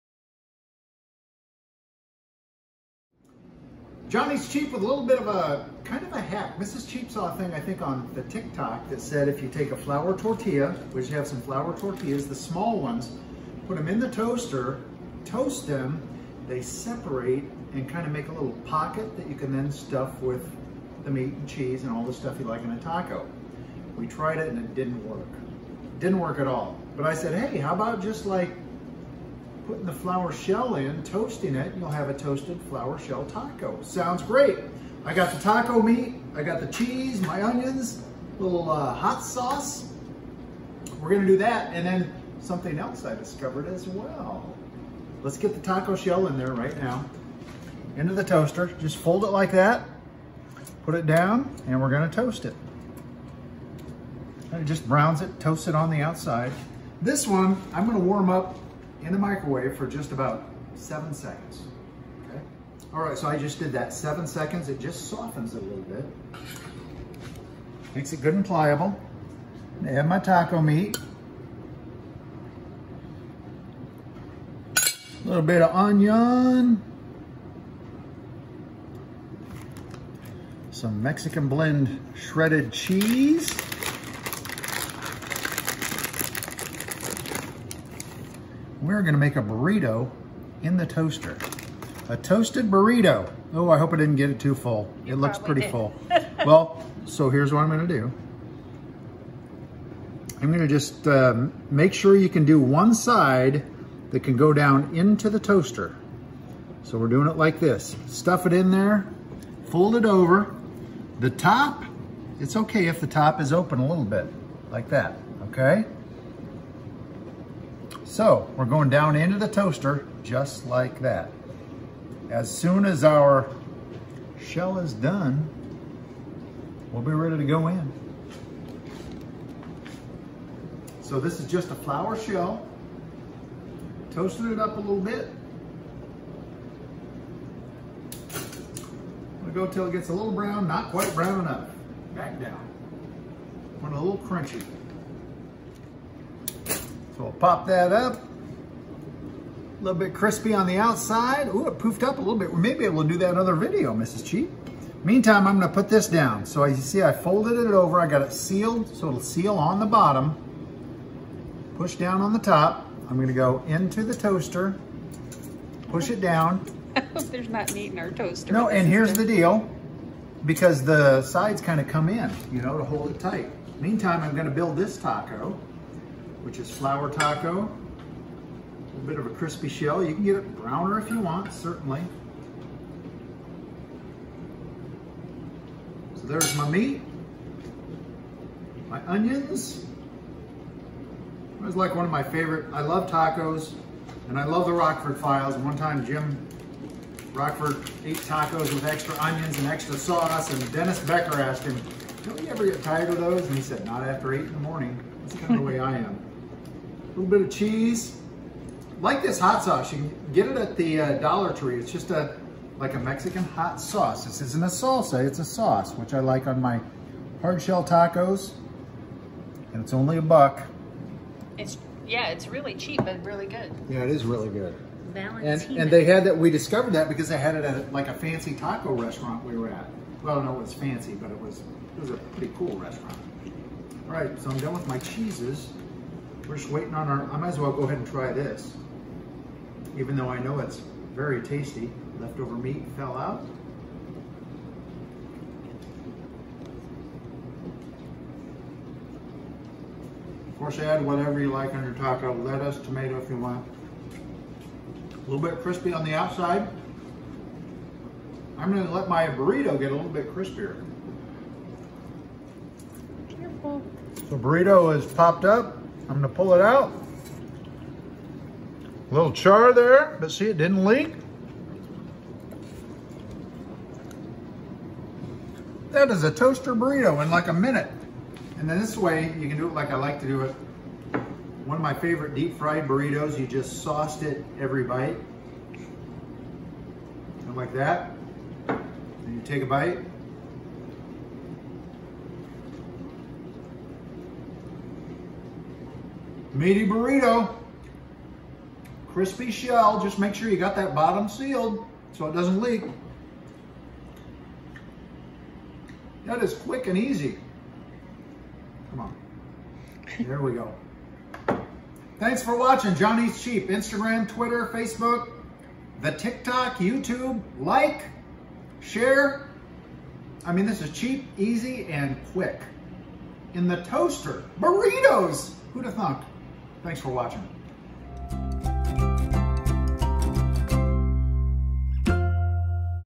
Crispy Toaster Burrito | Toaster Taco Shells | Easy Tacos!
John uses the toaster to make crispy flour tacos and toasty burritos. It’s quick and easy!